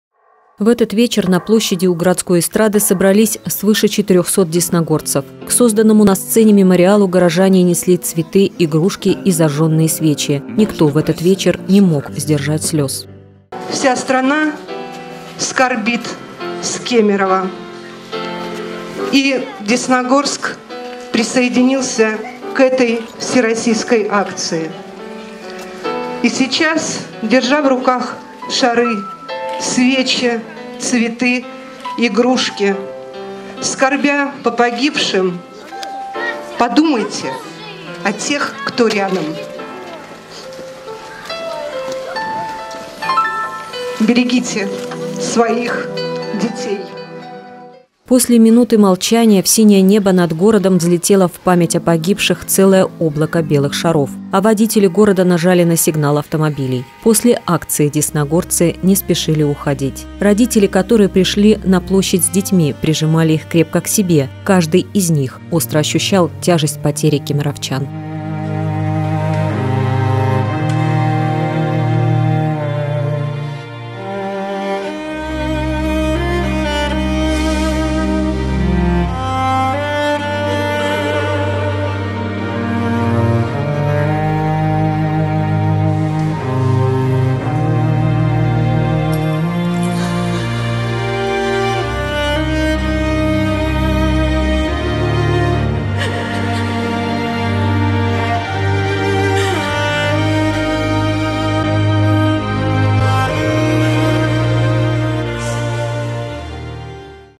Десна-ТВ: В Десногорске почтили память погибших в Кемерово
25 марта в торгово-развлекательном центре «Зимняя вишня» города Кемерово произошёл пожар, в результате которого погибли 64 человека из них 41 ребенок.  28 марта по всем городам нашей великой страны были приспущены флаги и прошла Общенациональная акция в память погибшим. Наш город тоже принял участие.